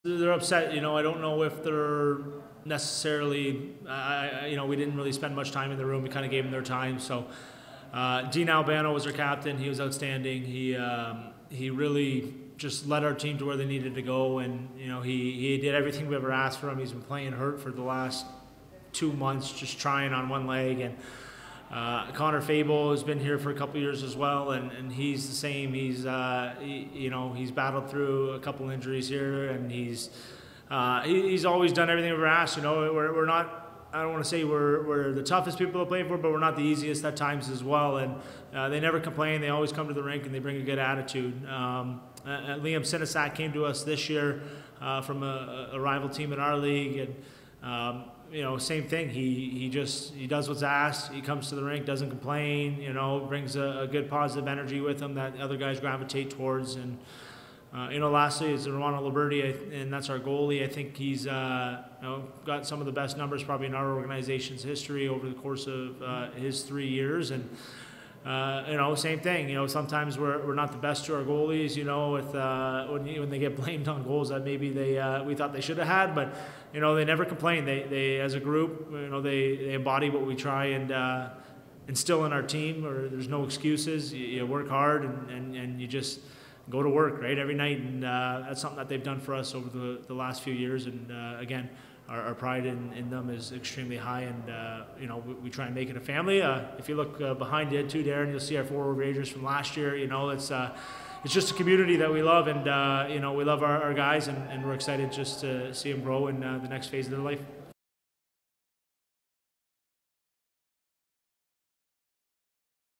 Lakeshore head coach recognizes his graduating players
Anthony Iaquinta spoke to the media after the Canadiens season ending loss on Saturday May 13th. During his news conference, Iaquinta took a few moments to recognize the efforts and contributions of his overage players.

Congrats to Lakeshore for another terrific season!